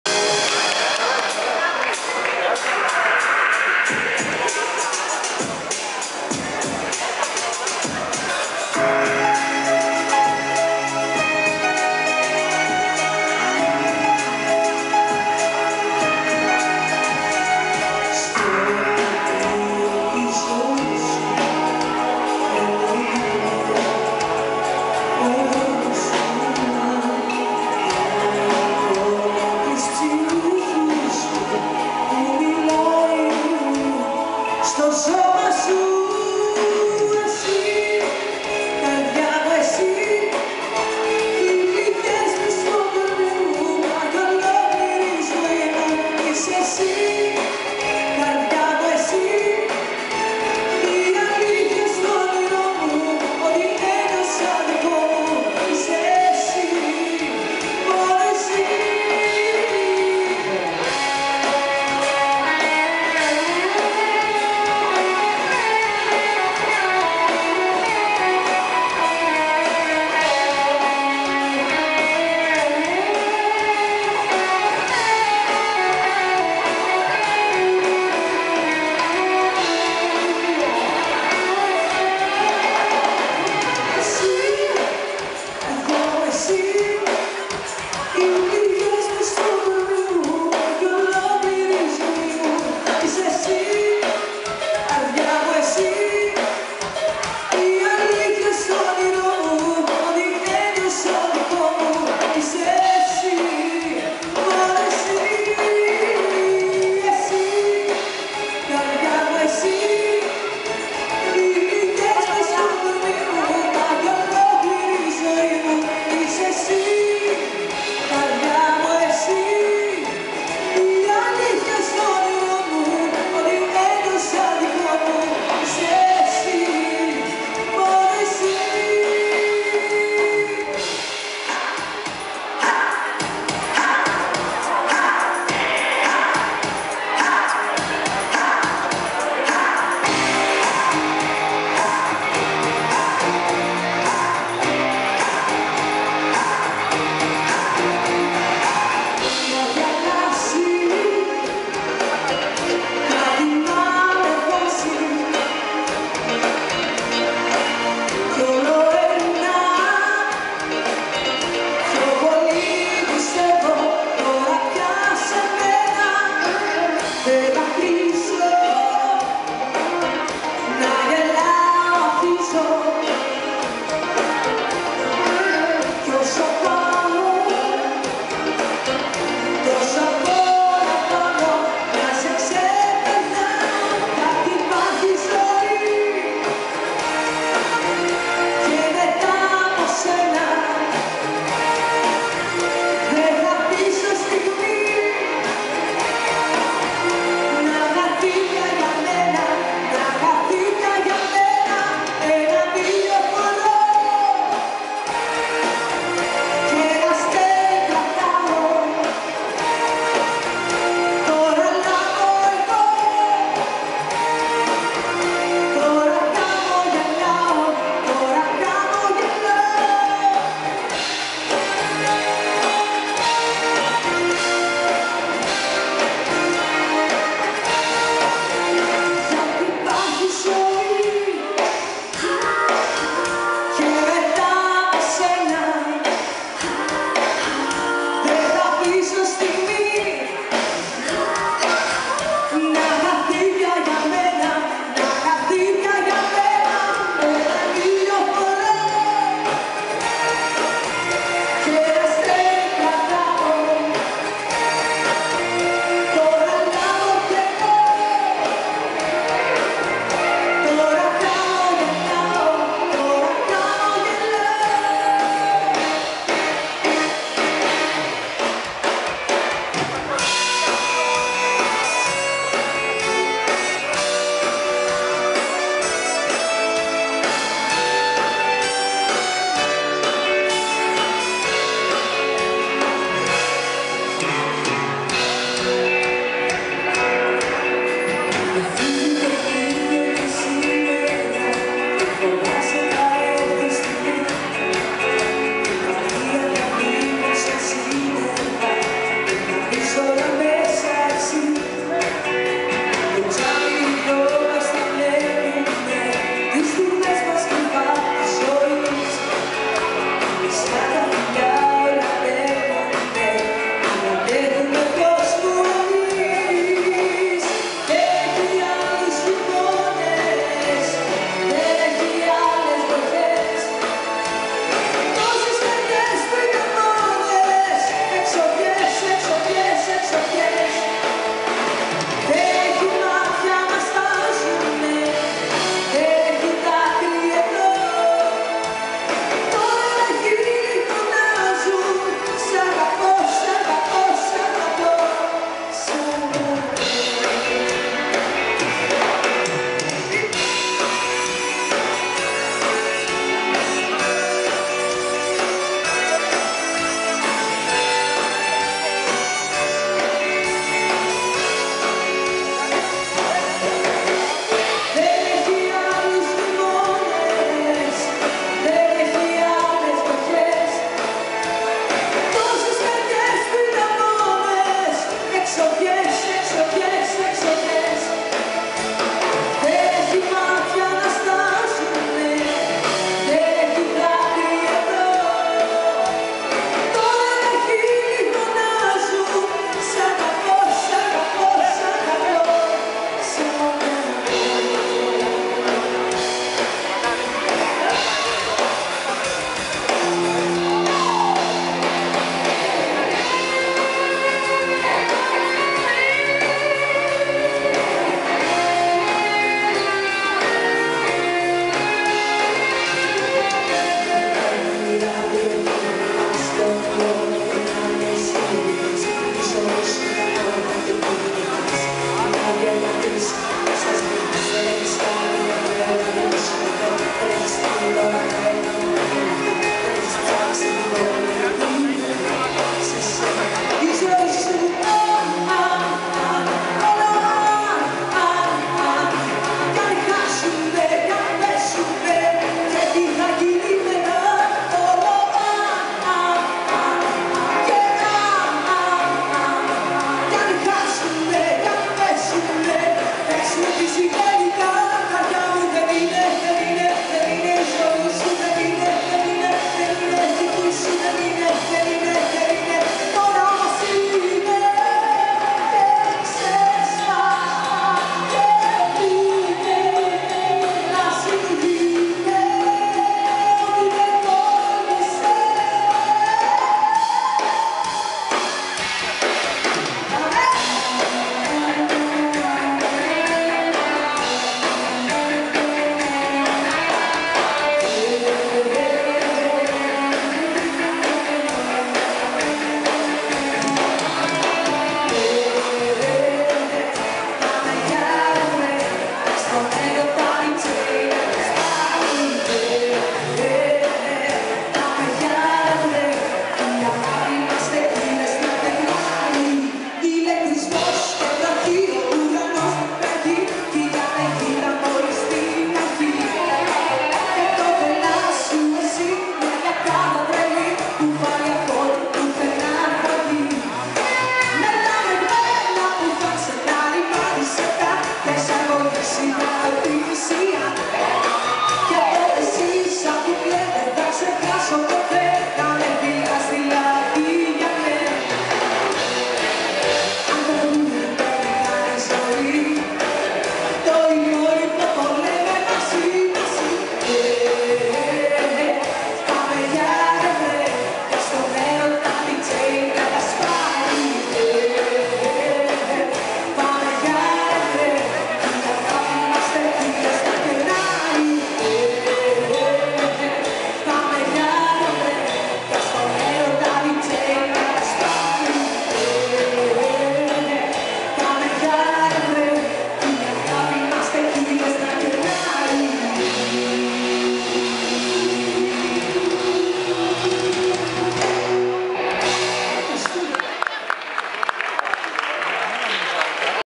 sofia danezi mamounia 2010
h sofia tragudai sta kalokairina mamounia tis thessalonikis ...sto proto tis programma ,,